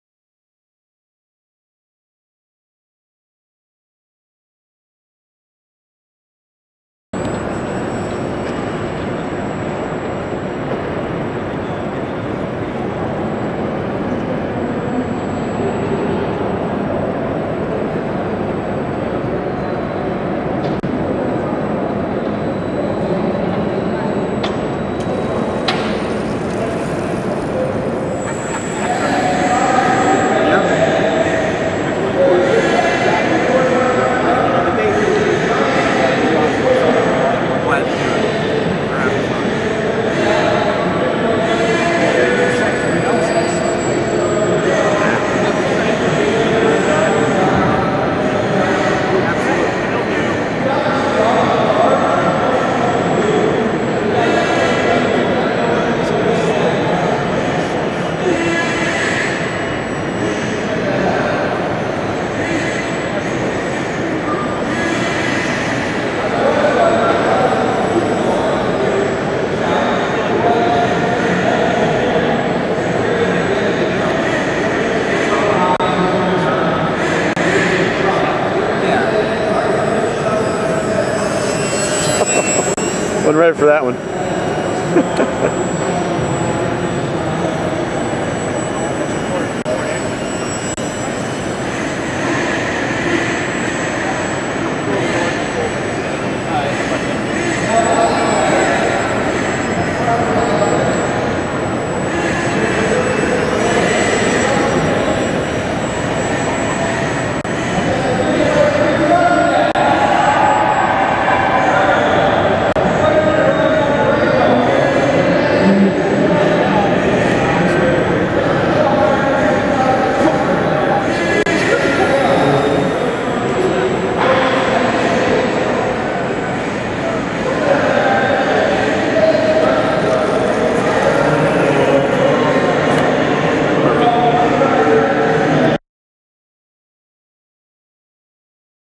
Hacker DH82 Tiger Moth Demo at IHobby 2012
23.5" wingspan, powered by an AX-1306N-2200 motor on a 2S 180 LiPo and a AR6400LBL.  Flight weight is 100.5g. It is sold as a free flight rubber powered kit that is easily converted to 3 channel RC.